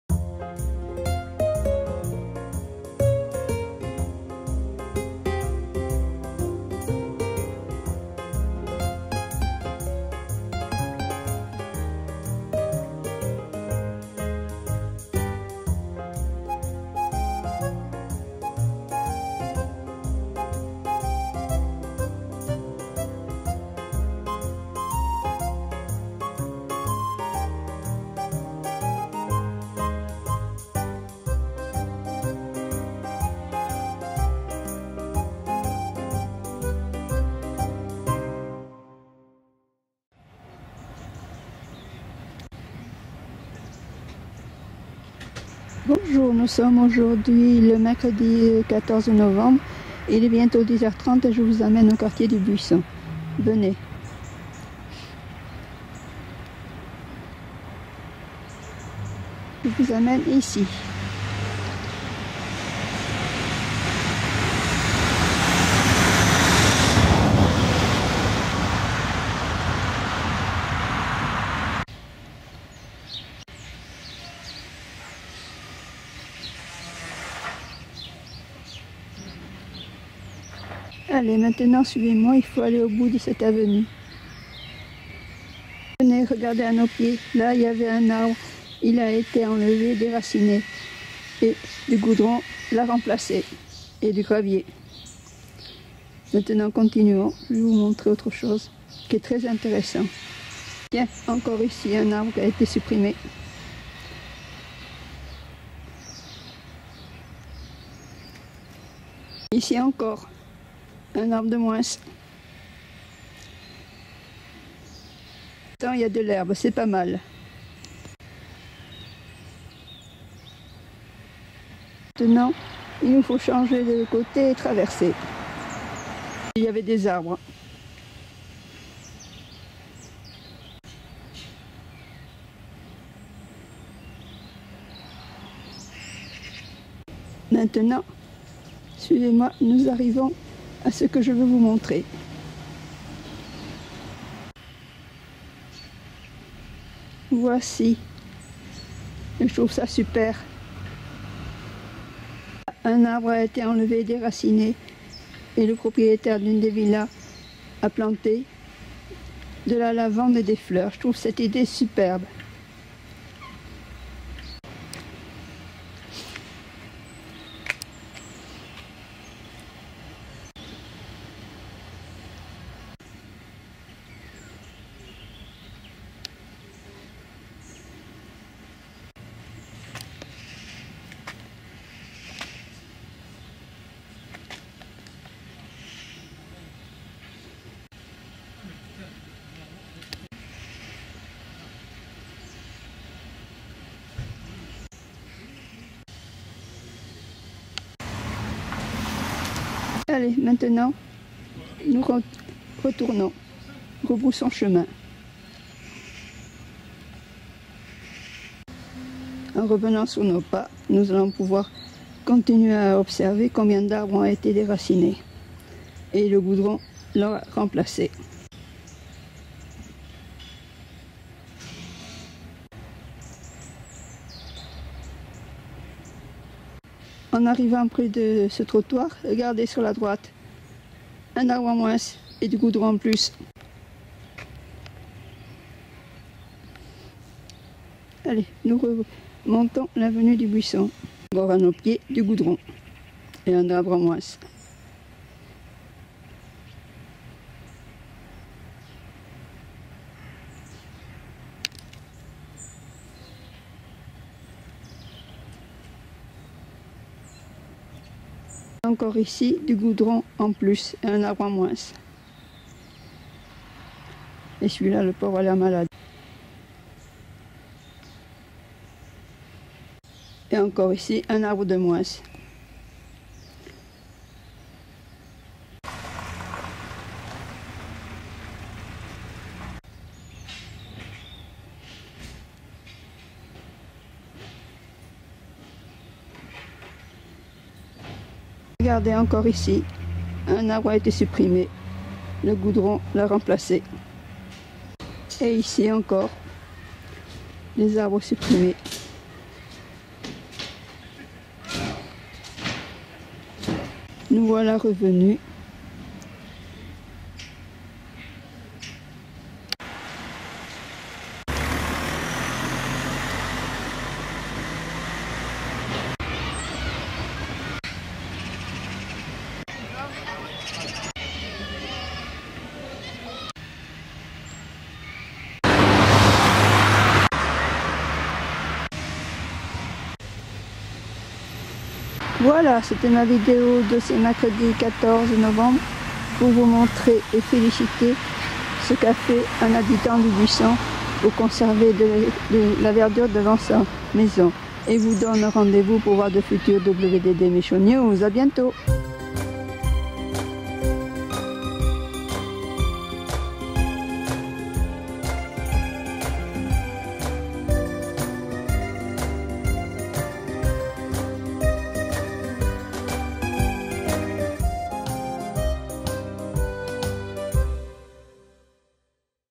LES W D D MICHOU NEWS 14 NOVEMBRE 2018 PAU FÉLICITATIONS À UN HABITANT DU BUISSON POUR LES PL
Bonjour,
pour avoir fait des plantations devant sa maison